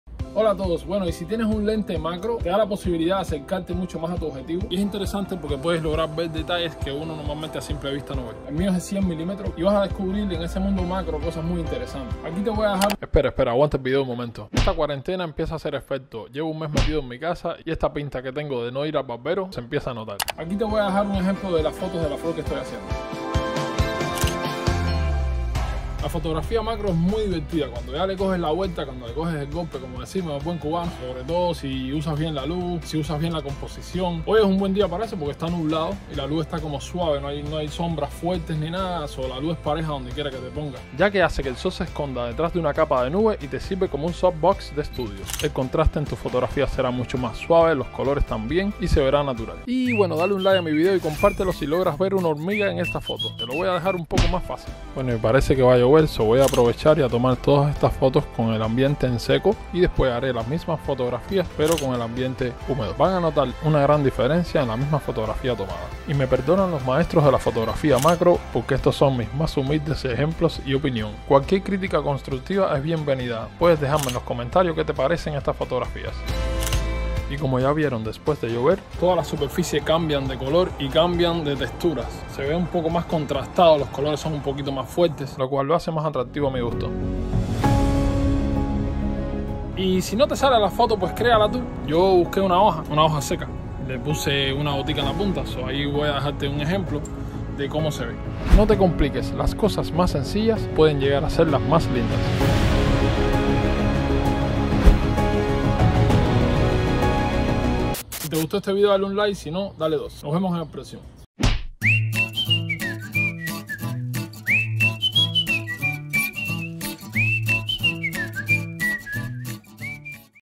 FOTOGRAFÍA MACRO FÁCIL DE HACER EN TU CASA O JARDÍN - ( EASY MACRO PHOTOGRAPHY TO DO AT HOME )
Macro photography with a Canon 100mm 2.8L lens.

FOTOGRAFIAS MACRO FACILES DE HACER EN TU CASA O JARDÍN.